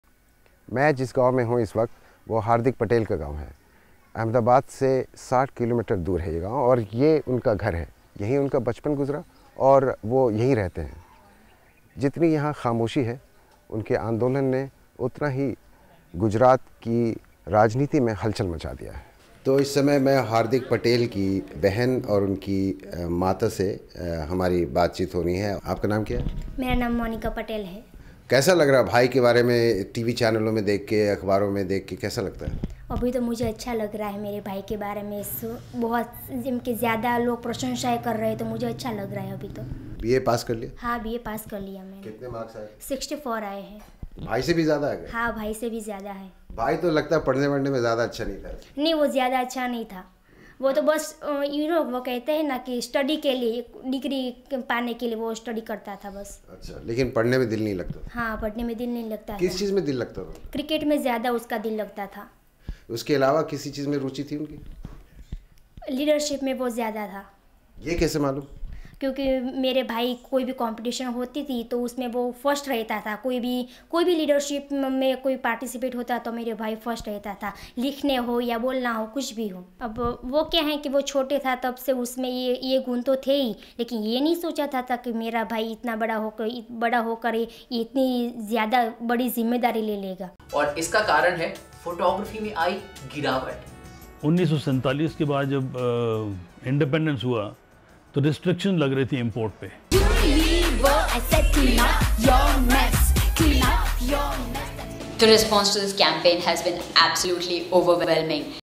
Hardik Patel's family on his agitation: BBC Hindi
गुजरात में पटेल आरक्षण आंदोलन की अगुआई करने वाले हार्दिक पटेल के गाँव पहुँचे बीबीसी संवाददाता ज़ुबैर अहमद. सुनिए हार्दिक की बहन क्या कहती हैं हार्दिक के इस आंदोलन के बारे में.
कैमरा- हेमंत कुशवाहा.